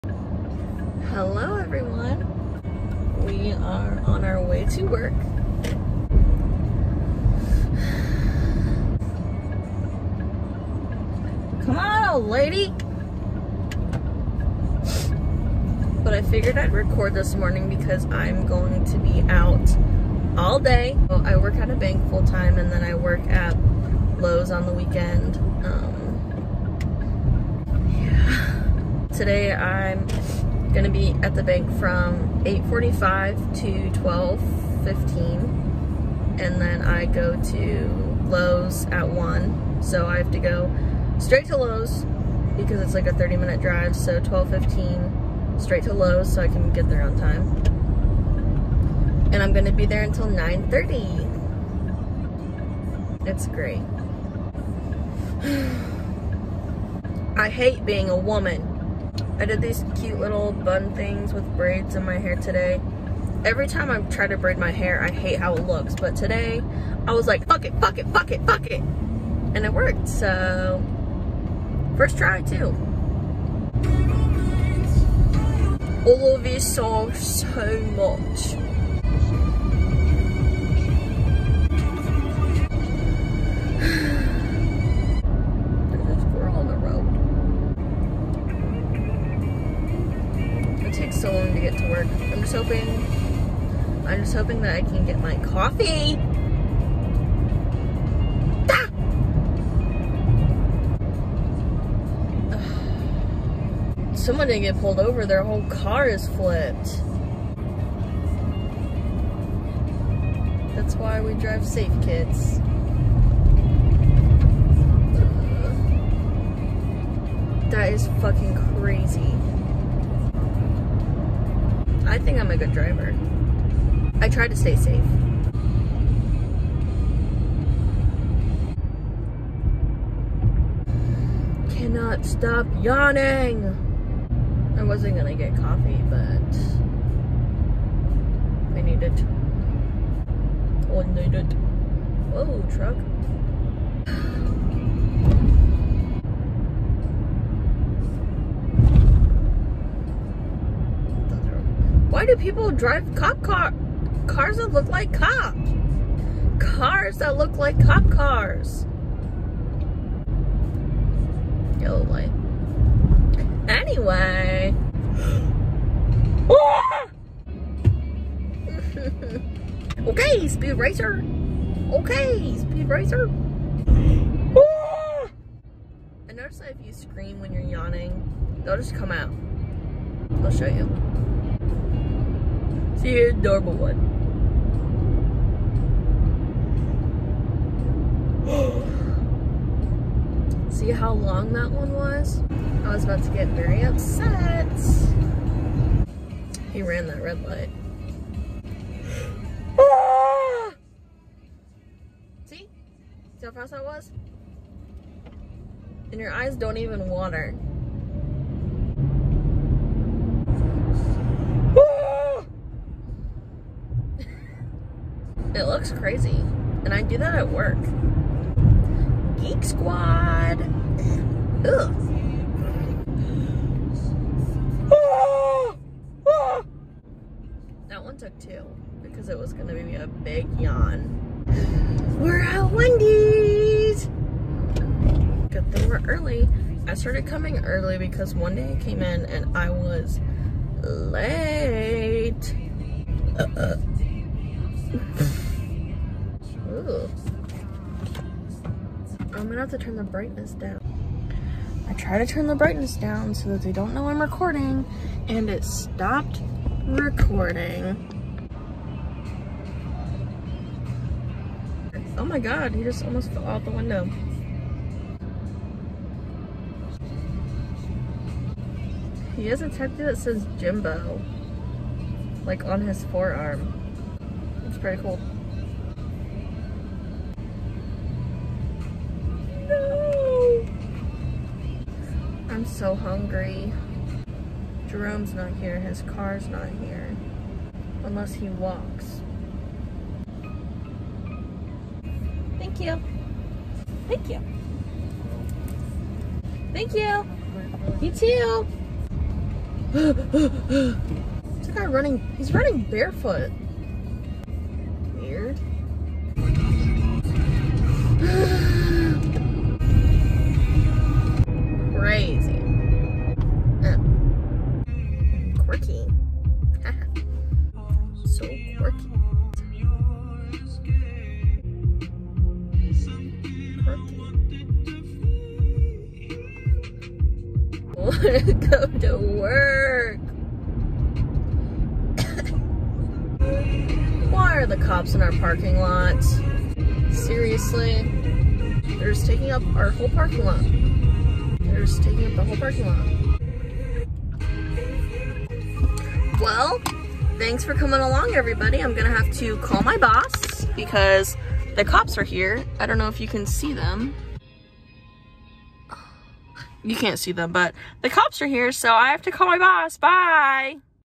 Driving to work #1
Driving to work, saw a car accident and the cops took up our whole parking lot at work. :(